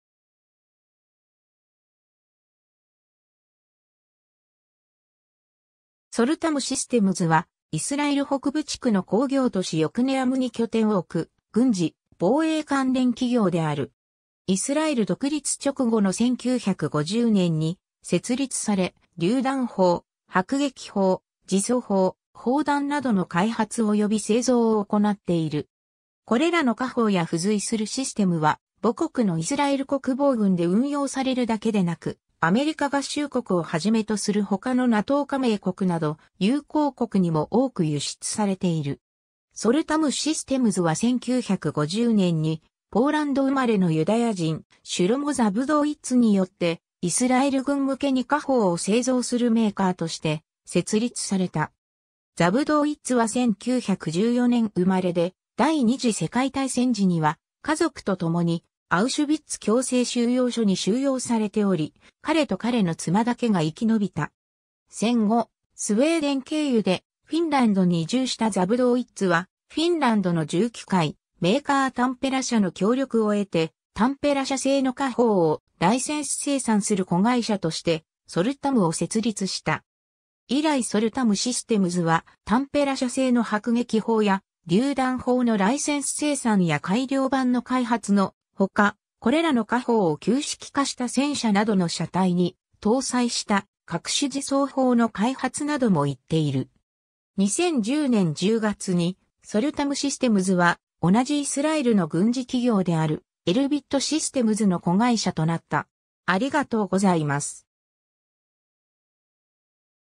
ソルタム・システムズ
北部地区_(イスラエル)
ソルタム・システムズ (英語: Soltam Systems, ヘブライ語: יָקְנְעָם עילית‎)は、イスラエル北部地区の工業都市ヨクネアムに拠点を置く軍事・防衛関連企業である。
イスラエル独立直後の1950年に設立され、榴弾砲・迫撃砲・自走砲・砲弾などの開発および製造を行っている。
これらの火砲や付随するシステムは母国のイスラエル国防軍で運用されるだけでなく、アメリカ合衆国をはじめとする他のNATO加盟国など、友好国にも多く輸出されている。
 ソルタム・システムズは1950年にポーランド生まれのユダヤ人シュロモ・ザブドウィッツによって、イスラエル軍向けに火砲を製造するメーカーとして設立された。
 ザブドウィッツは1914年生まれで、第二次世界大戦時には家族と共にアウシュヴィッツ強制収容所に収容されており、彼と彼の妻だけが生き延びた。
戦後、スウェーデン経由でフィンランドに移住したザブドウィッツはフィンランドの重機械メーカータンペラ社の協力を得て、タンペラ社製の火砲をライセンス生産する子会社としてソルタムを設立した。
 以来ソルタム・システムズは、タンペラ社製の迫撃砲や榴弾砲のライセンス生産や改良版の開発の他、これらの火砲を旧式化した戦車などの車体に搭載した各種自走砲の開発なども行っている。
 2010年10月に、ソルタム・システムズは同じイスラエルの軍事企業であるエルビット・システムズの子会社となった。